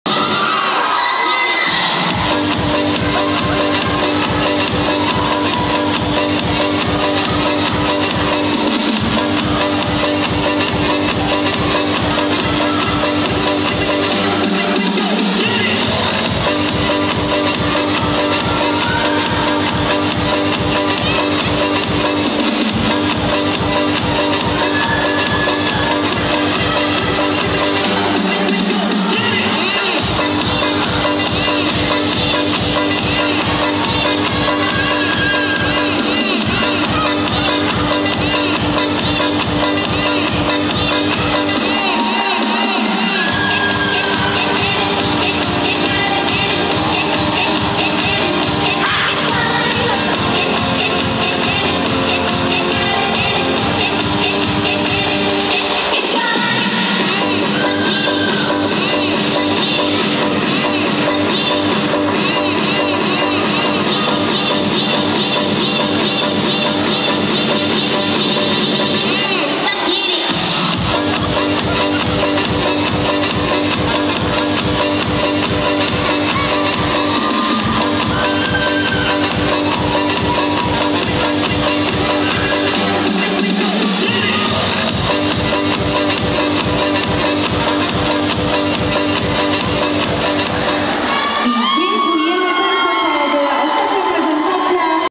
Explosion Cup DD Małe Grupy Dzieci Vulcan Piastów 4M
Explosion Cup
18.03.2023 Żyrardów

Disco Dance Małe Grupy Dzieci Śr. zaaw./ zaawansowani
Vulcan Piastów  4 miejsce
Vulcan Piastów